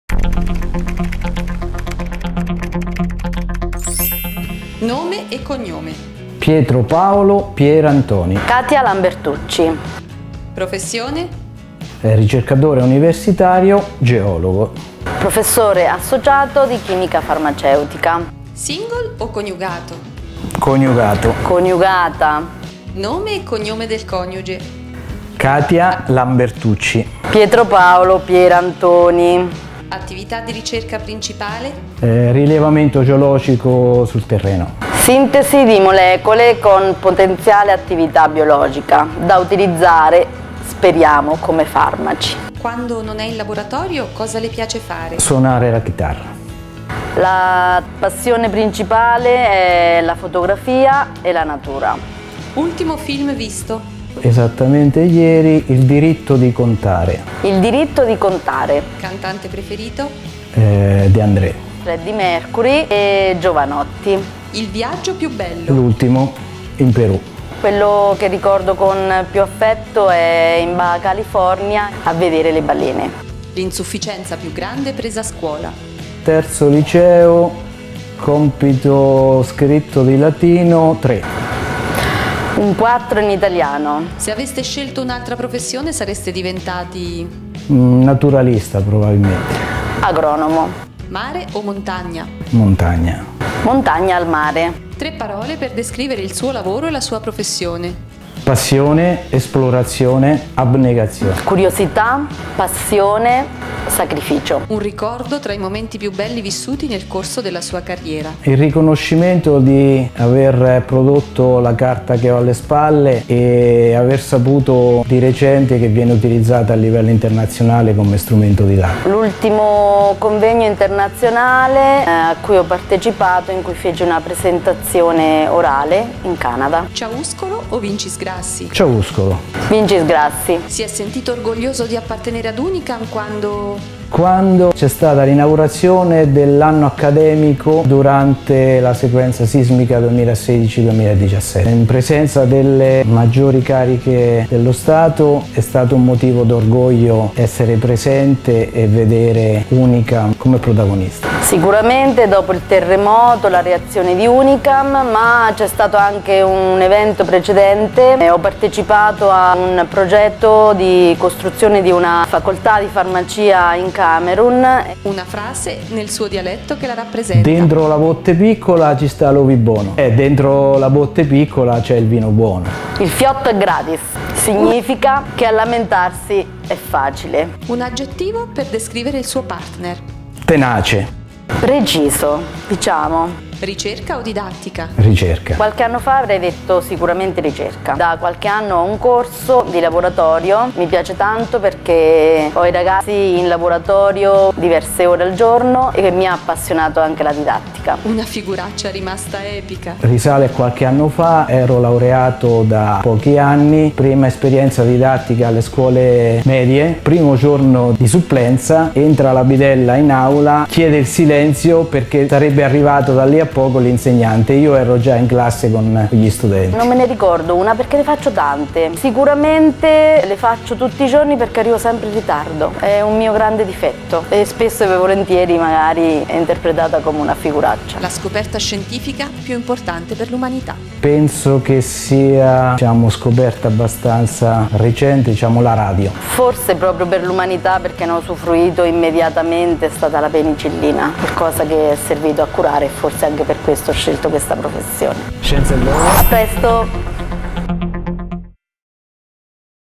Ritratto di scienziato - Intervista doppia Lambertucci vs Pierantoni
Ritratto di scienziato - Scienza e Lode
Catia Lambertucci vs Pietro Paolo Pierantoni